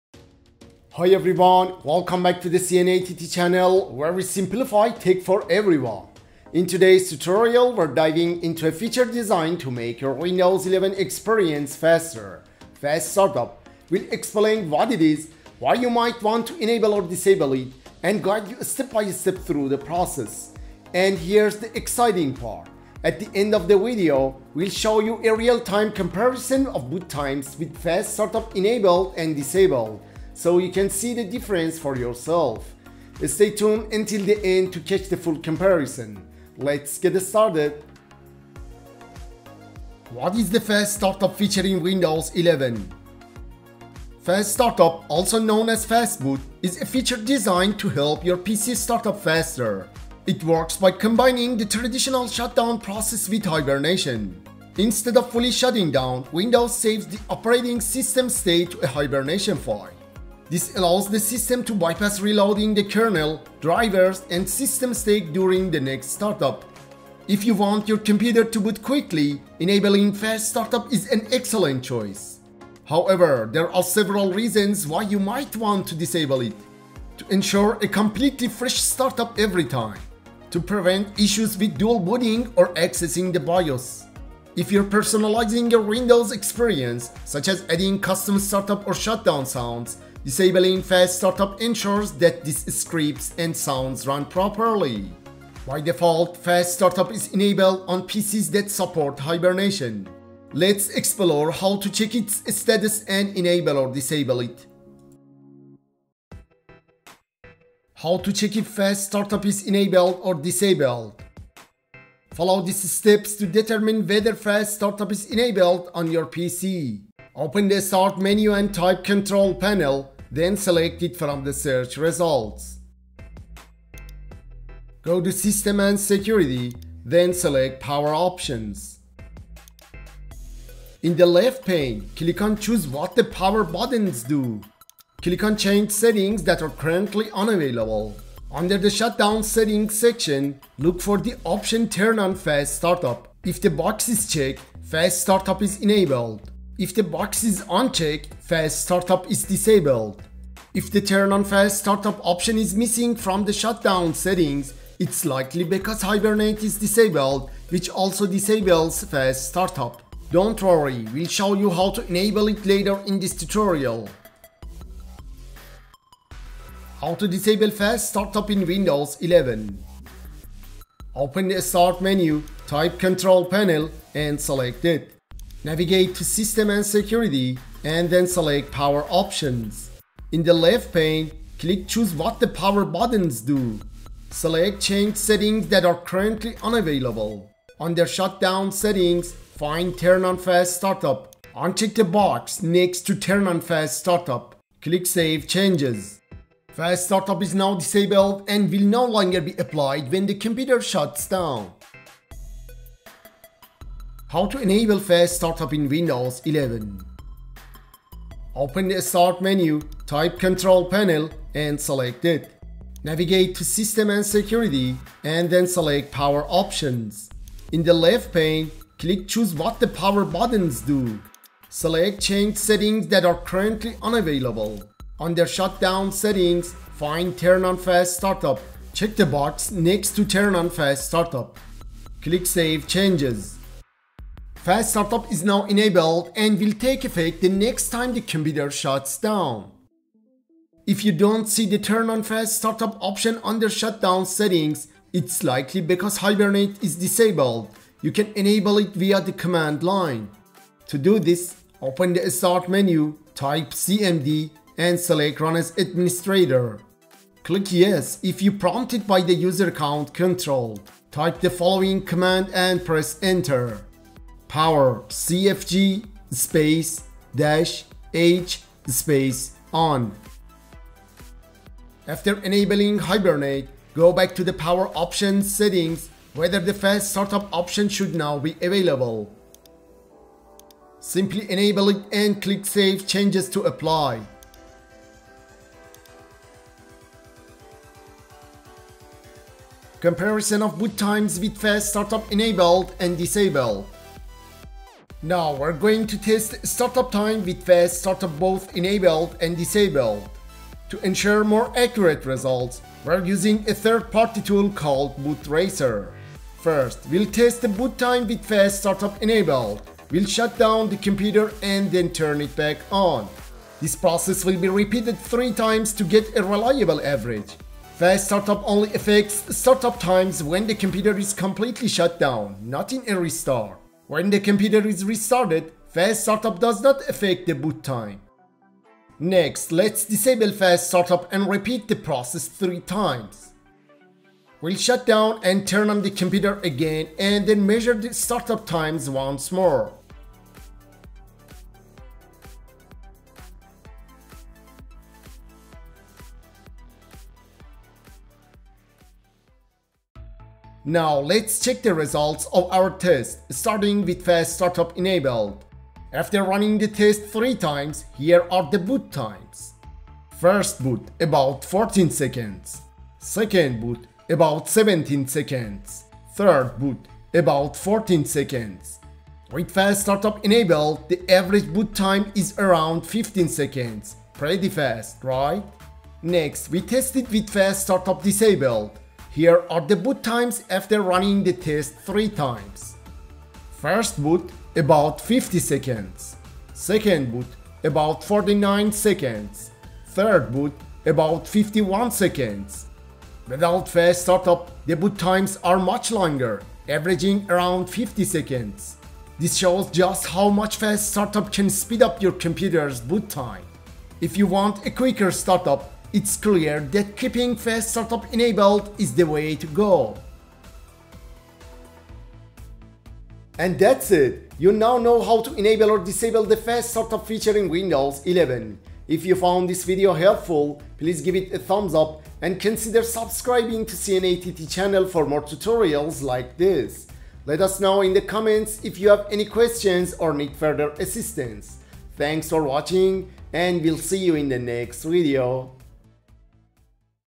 How to Enable or Disable Fast Startup in Windows 11
In this tutorial, we’ll explore the Fast Startup feature in Windows 11, explaining how it works, its benefits, and why you might want to enable or disable it.

📌 **Timestamps:**
00:00 Introduction
00:39 What is Fast Startup?
01:47 How to Check Fast Startup Status
02:46 How to Disable Fast Startup
03:23 How to Enable Fast Startup
04:49 Boot Time Comparison: Enabled vs. Disabled

You'll learn how to:
✔️ Check if Fast Startup is enabled or disabled
✔️ Enable Fast Startup step-by-step
✔️ Disable Fast Startup step-by-step
✔️ Use the command line to enable Hibernate (if required)

We’ve also tested and compared boot times with Fast Startup enabled and disabled to show you the impact this feature can have on your PC’s performance.

Let us know your thoughts or questions in the comments below!